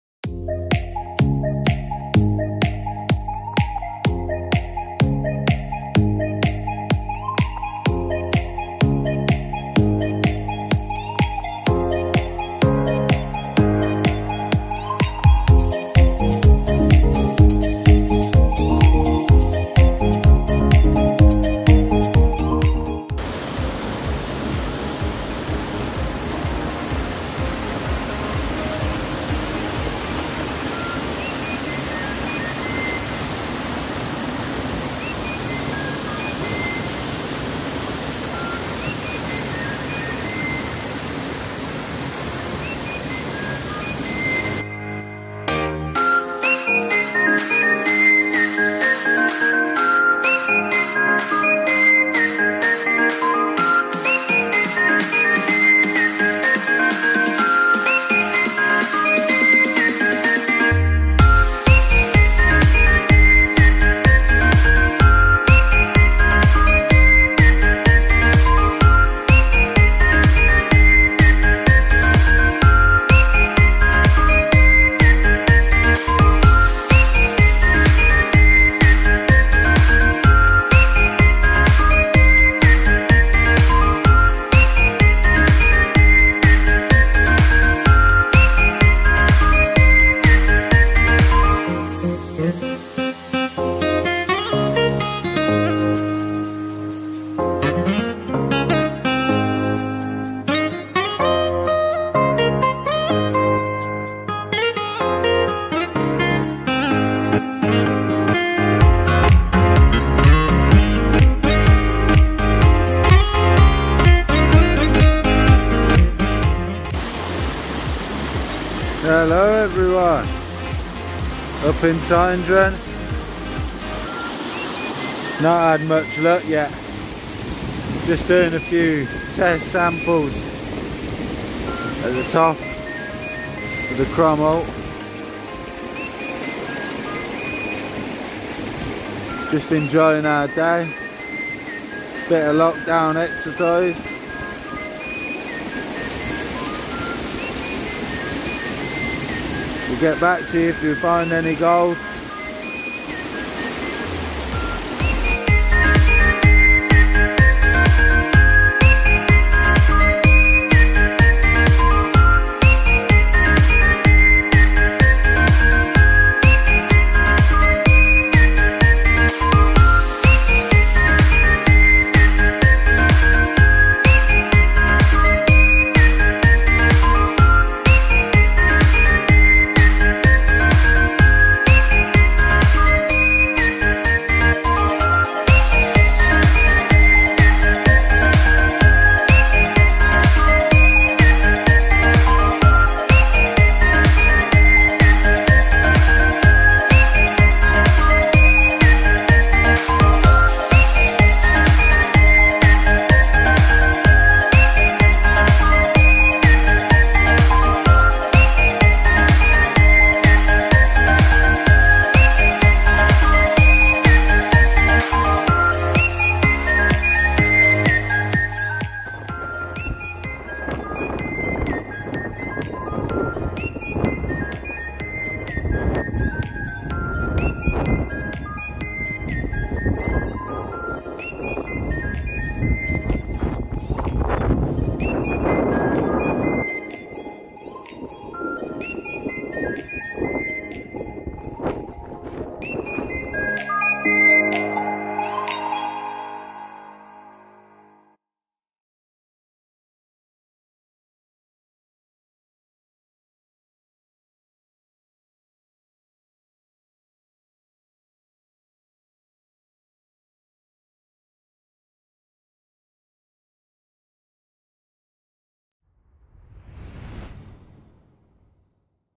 Exploring Crom Allt Burn Tyndrum. Walking from the Bottom to the Top with Stunning views!
A great day out Exploring the Crom Allt Burn in Tyndrum. We walked from the bottom to the top taking test pans with mixed results. We soon ditched all the gear and headed up to the top. We didn't stay long but nevertheless it was a great adventure.